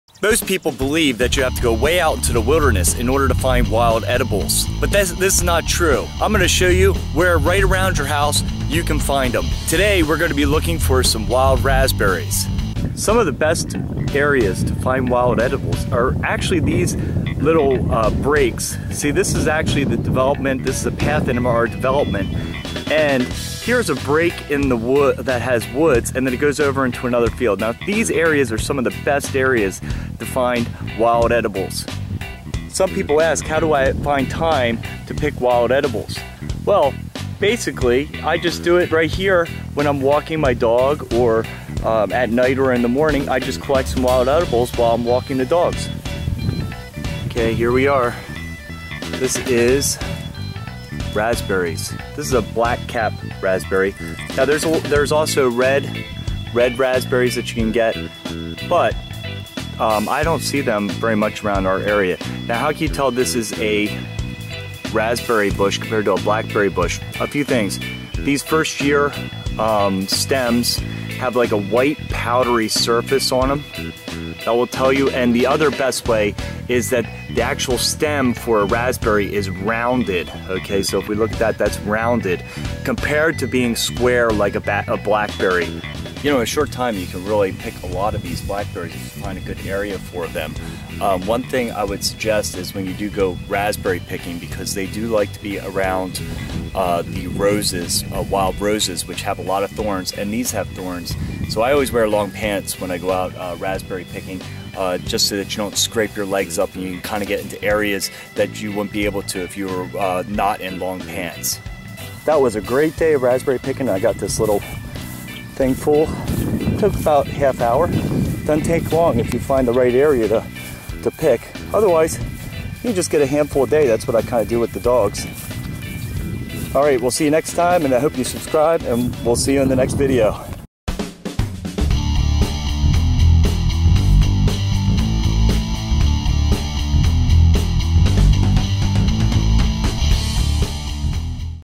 Summer Wild Raspberry Picking
Who says that you can't find wild edibles right around the home! I found that a great place to look is the transition areas with a break of trees. Today, I am picking wild raspberries.

You may ask yourself, "How to I find time to pick wild edibles?" Well, the answer is to make it part of your normal routine. For example, I like to pick some while I walk my dogs.

Even if you pick only a handful a day, you are on your way to a more healthy and natural lifestyle. You can always scale up your activity in a emergency situation.

In this video, I am picking Black capped Wild Raspberries. You can easily identify these by the round white powdered thorn stem and the berries.

I wear long pants for raspberry picking because of the torns and the fact that they tend to be in areas with thorny wild roses.

Happy Picking!